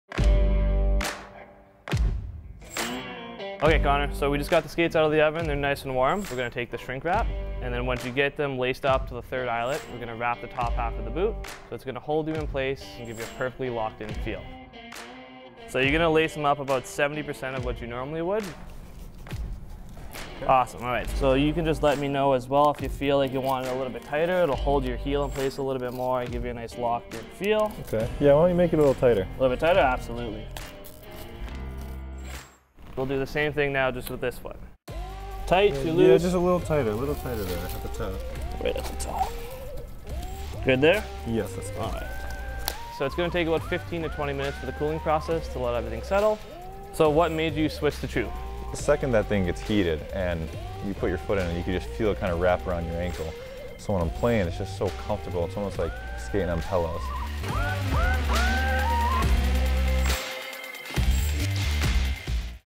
Connor Hellebuyck Get's His TRUE PRO Custom Skates
With our scans, we design a 3D mold of your foot and custom build your skate from the inside out. Final baking and wrap seals the shape, giving you best-in-class fit and next-level performance. Check out that white toe cap on Winnipeg Jets goalie and TRUEPRO Connor Hellebuyk's new rockets.